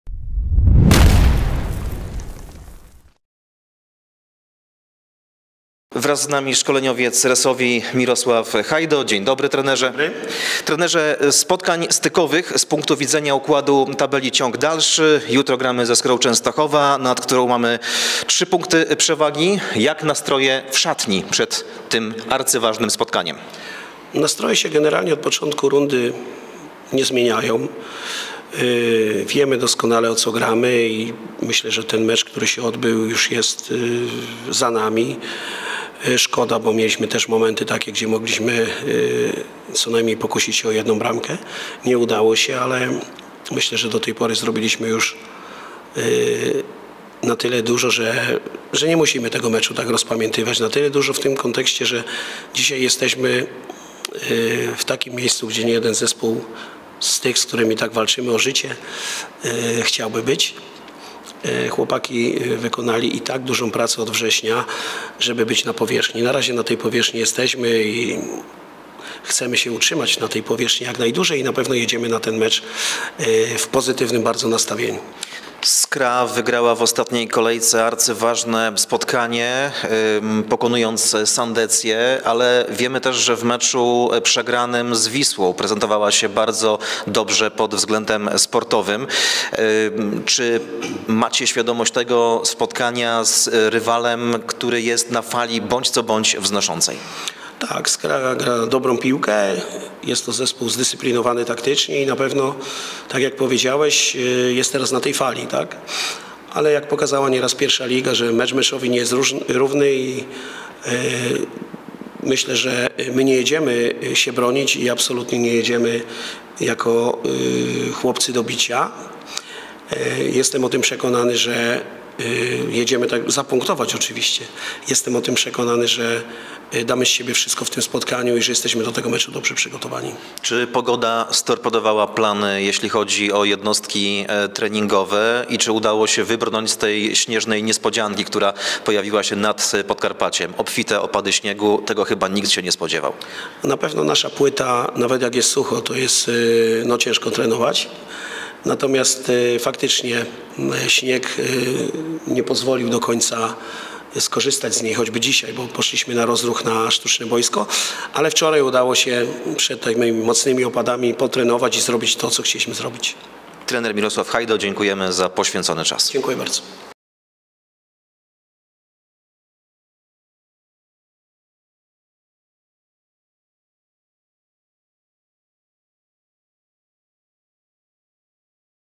PRZED MECZEM ZE SKRĄ CZĘSTOCHOWA | MIROSŁAW HAJDO
Przed sobotnim meczem 26. kolejki Fortuna 1 Liga przeciwko Skrze Częstochowa porozmawialiśmy z trenerem Mirosławem Hajdo.

Fotopoker.pl wspiera Resovia TV